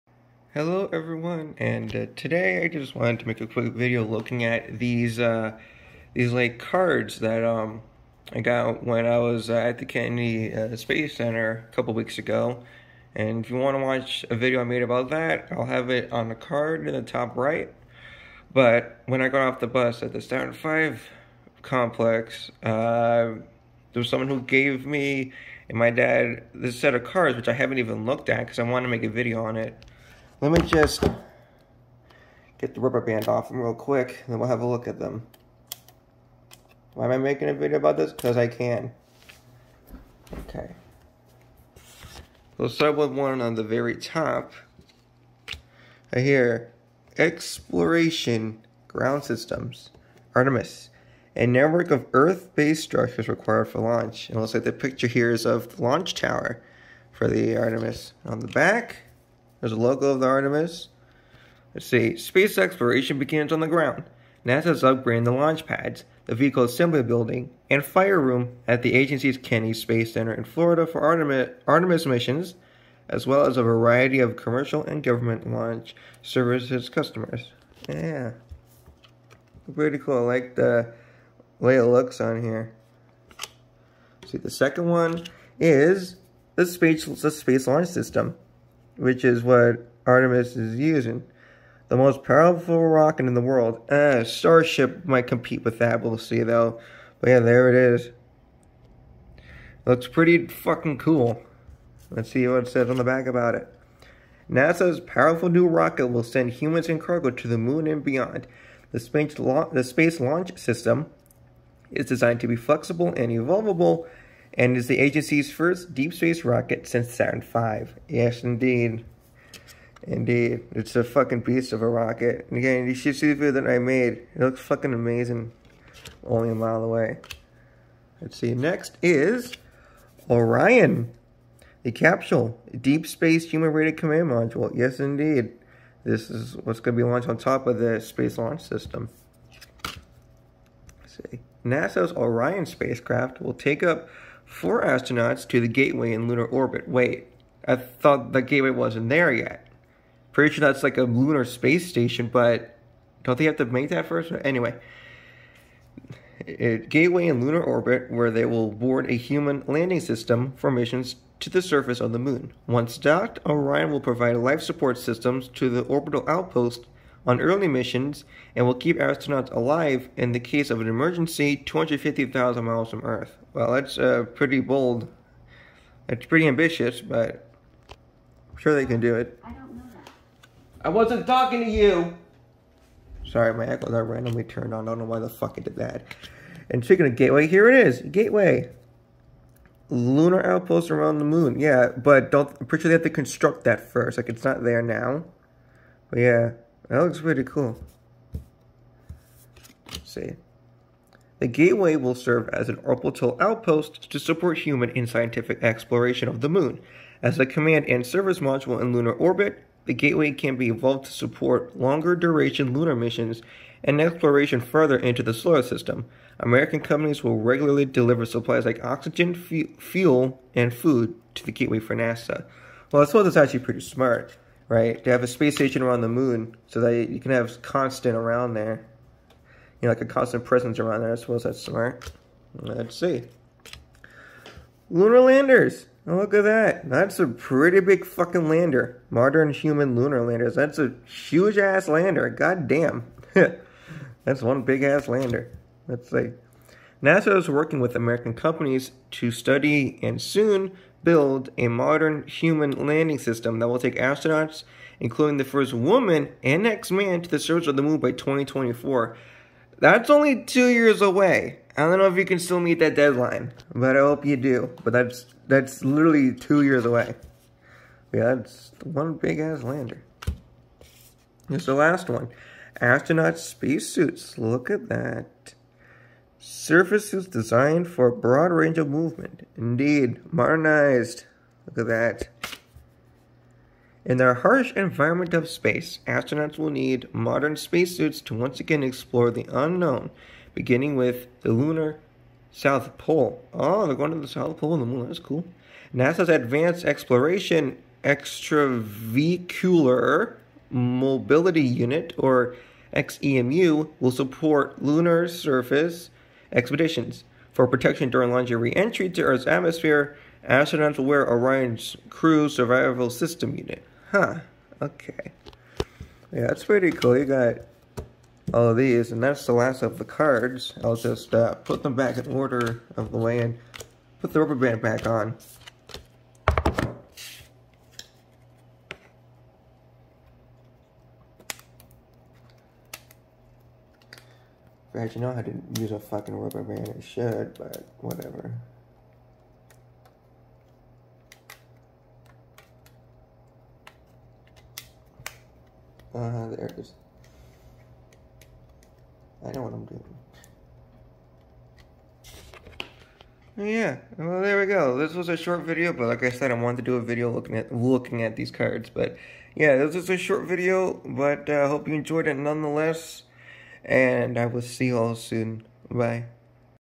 Looking at some NASA Artemis Cards I got during my trip to the Kennedy Space Center on 04/07/2022
Just looking at some cards that I got during my trip to the Kennedy Space Center on April 7th, 2022.